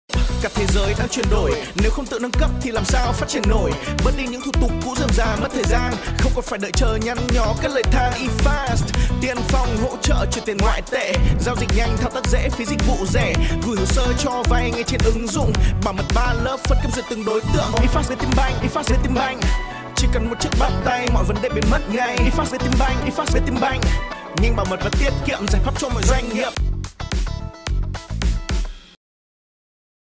VietinBank eFAST - Ngân hàng số của doanh nghiệp
✅ Trợ lý tài chính số đáp ứng nhanh chóng mọi nhu cầu giao dịch của doanh nghiệp.
✅ Công nghệ AI - Bigdata kết hợp Machine learning hỗ trợ doanh nghiệp đưa ra quyết định kinh doanh nhanh chóng, chuẩn xác.
✅ Nền tảng mở (Open Banking) tối ưu hóa và nâng cao trải nghiệm dịch vụ, đồng hành cùng doanh nghiệp trong giai đoạn chuyển đổi số.
🎁 Miễn 100% phí chuyển tiền trong hệ thống, liên ngân hàng, phí nộp lương, nộp NSNN… đối với khách hàng kích hoạt mới.